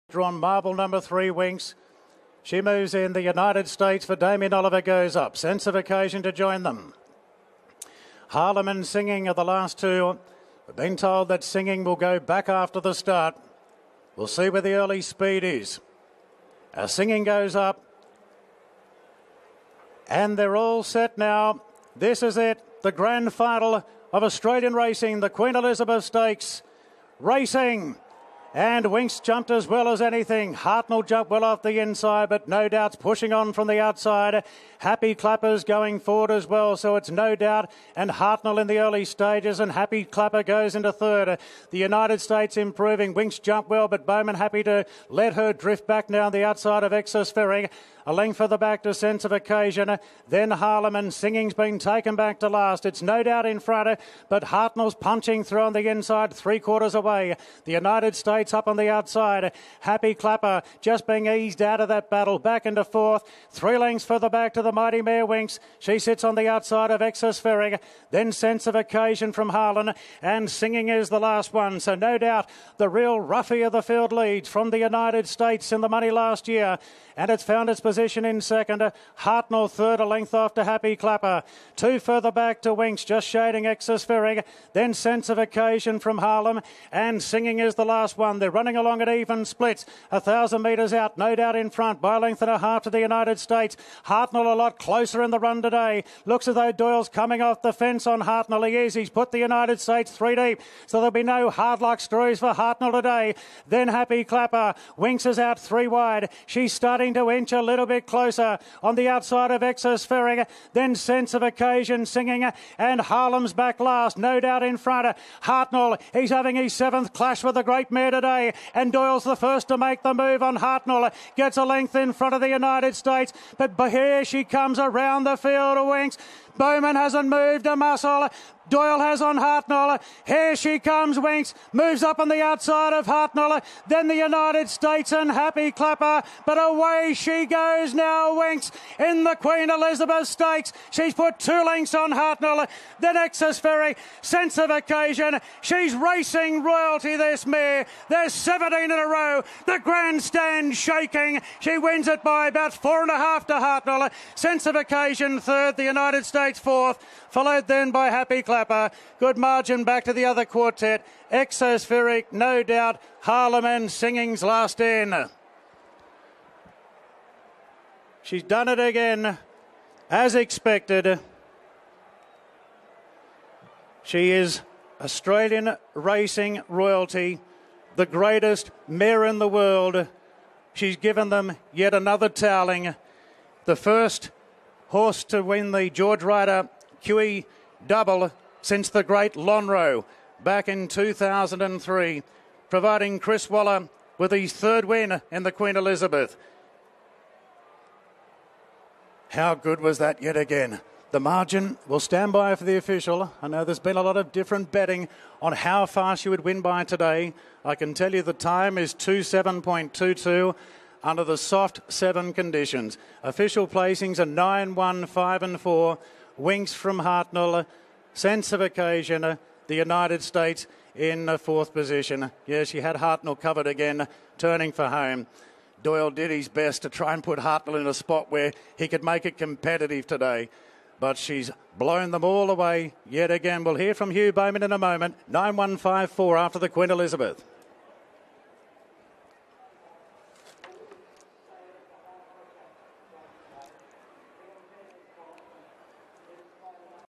Winx (AUS) - 2017 Longines Queen Elizabeth Stakes
The 2017 Longines Queen Elizabeth Stakes goes to Winx for her 17th consecutive victory.